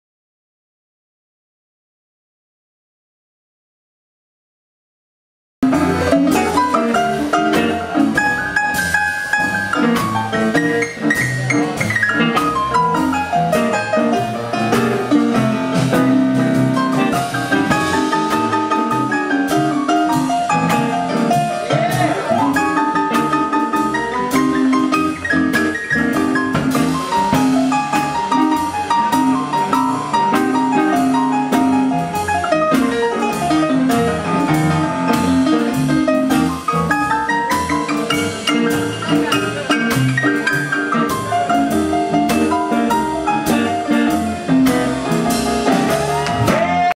Big Joe and The Dynaflows featuring Red Young
Red Young solo during show at Loafers Beach Club, Raleigh, NC.